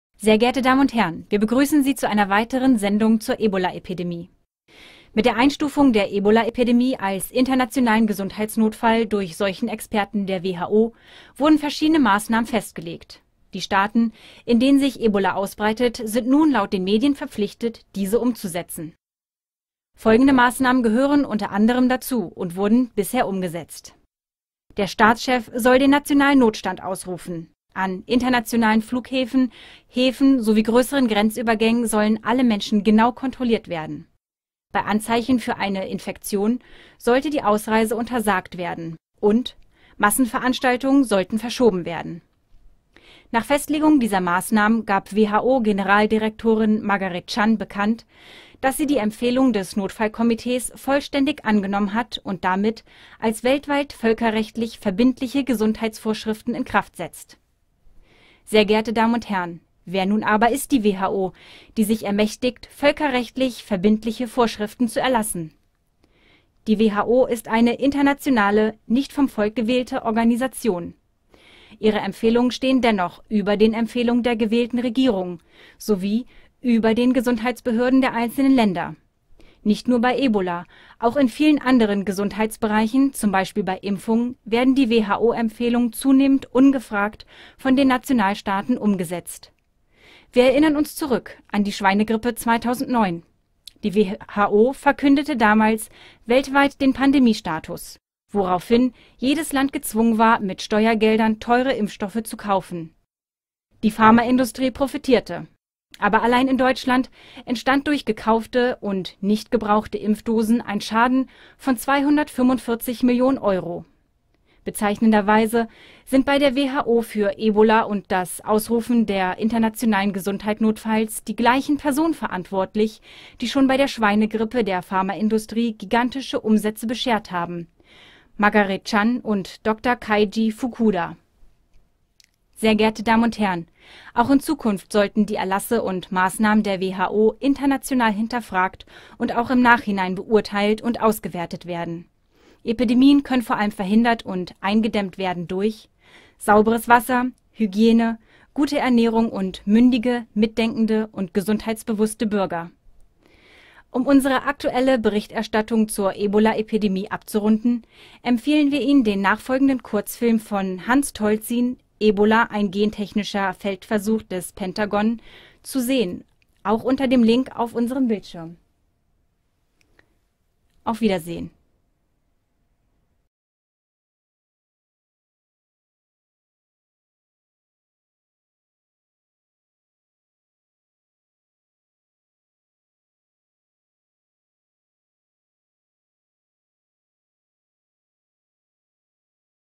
Die Ebola-Epidemie und die Rolle der WHO?
Mit der Einstufung der Ebola-Epidemie als internationalen Gesundheitsnotfall durch Seuchenexperten der WHO, wurden verschiedene Massnahmen festgelegt. Die Staaten, in denen sich Ebola ausbreitet, sind nun laut den Medien verpflichtet, diese umzusetzen.

Folgende Massnahmen gehören unter anderem dazu und wurden bisher umgesetzt:
- der Staatschef soll den nationalen Notstand ausrufen
- an internationalen Flughäfen, Häfen sowie grösseren Grenzübergängen sollen alle Menschen genau kontrolliert werden. Bei Anzeichen für eine Infektion sollte die Ausreise untersagt werden.
- Massenveranstaltungen sollten verschoben werden.